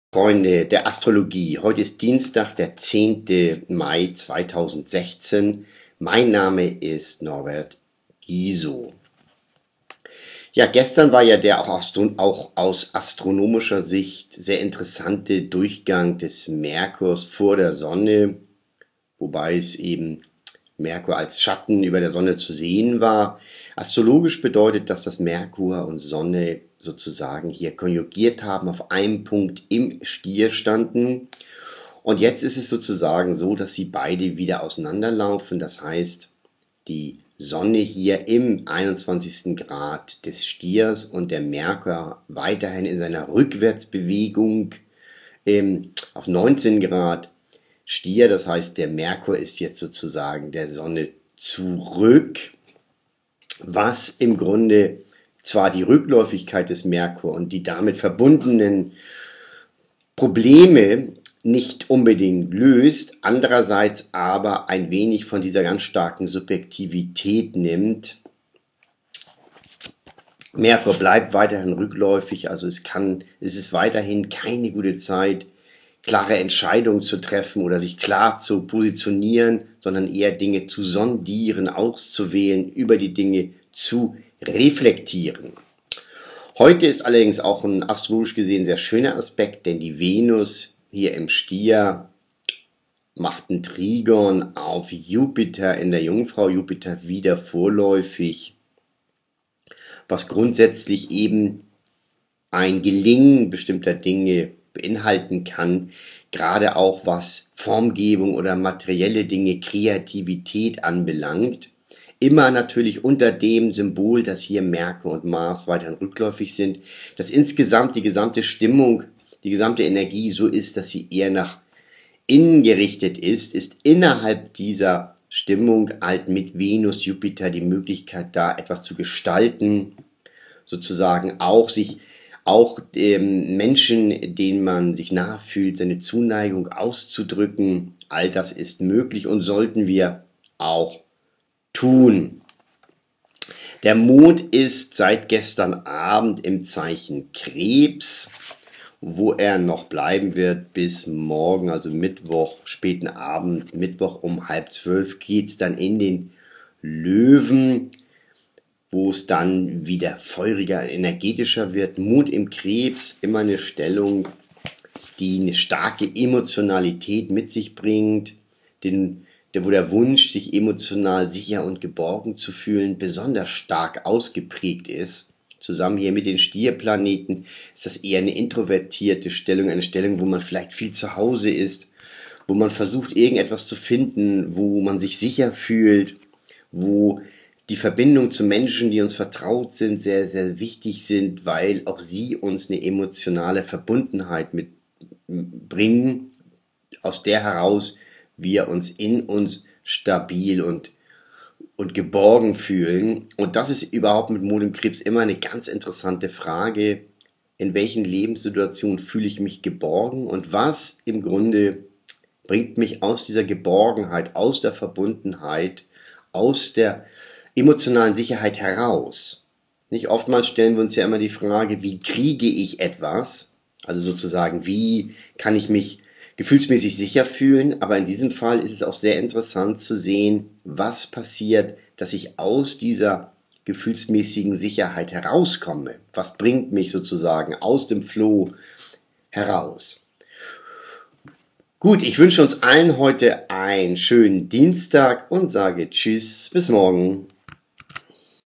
Tageshoroskop Dienstag 10.Mai 2016
und präsentieren das Tageshoroskop vom 10. Mail 2016 interpretiert vom Astrologen Norbert Giesow (Tel.0431-94688). Der Tag nach der Konjunktion von Merkur und Sonne, Venus und Jupiter haben einen schönen Aspekt, der Mond ist im Krebs, mehr dazu im Video...